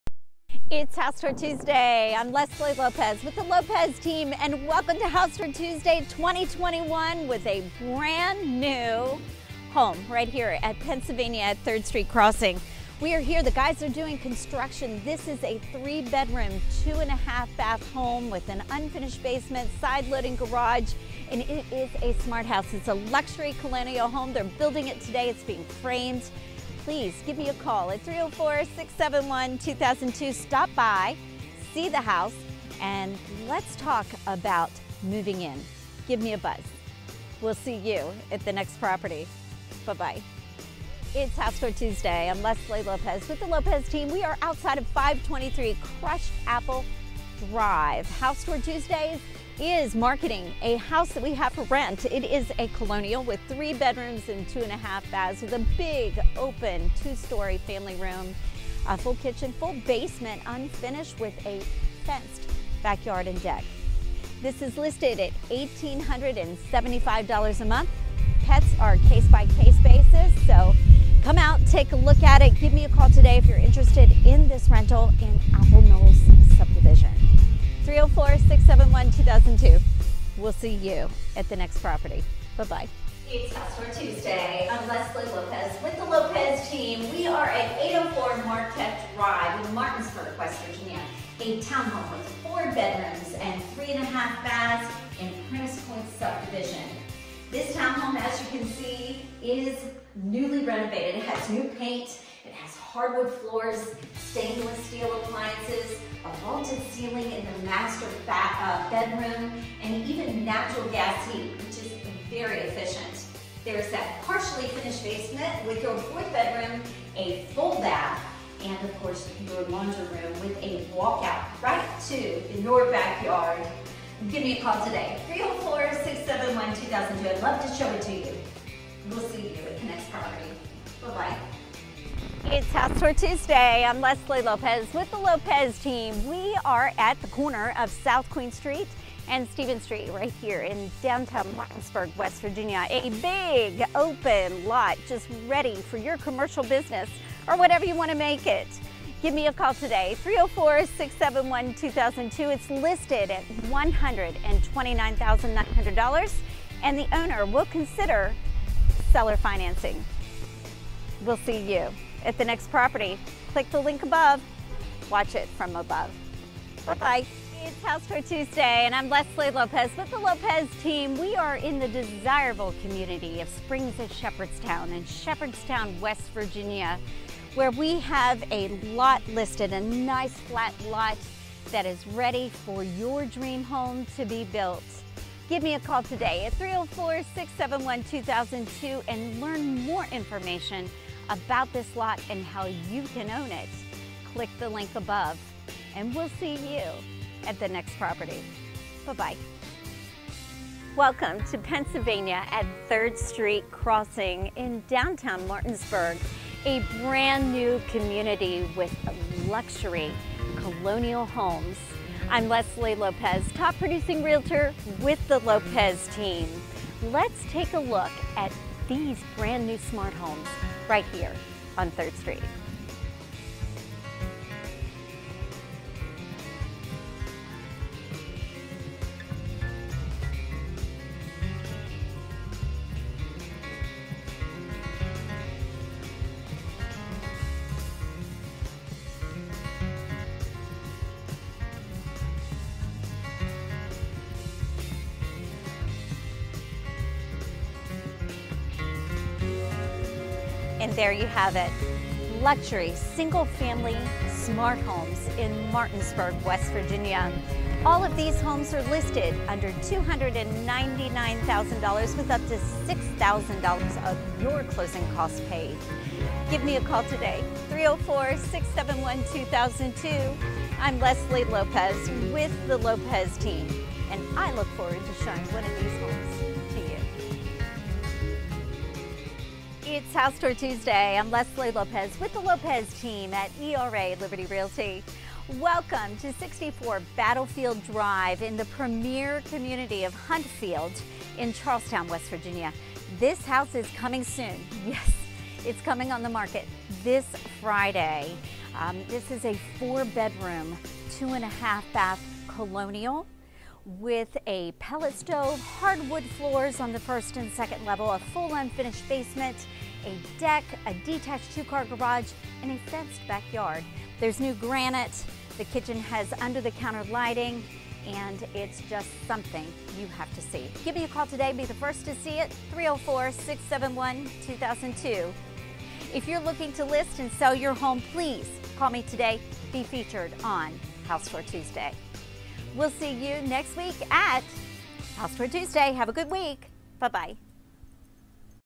It’s HOUSE TOUR TUESDAY! January 5, 2021
It's HOUSE TOUR TUESDAY! On the Hour... Every Tuesday... Starting at 10 AM ... on Facebook, Instagram, YouTube and Twitter! Join Top Producing Realtor, Lesley Lopez as she takes you to listings she has For Sale in WV. WV0024639 - John Kilroy, Broker @thelopezteamwv @lesleylopezrealtorwv thelopezteamwvsellshomes HouseTourTuesday SEE YOU THERE!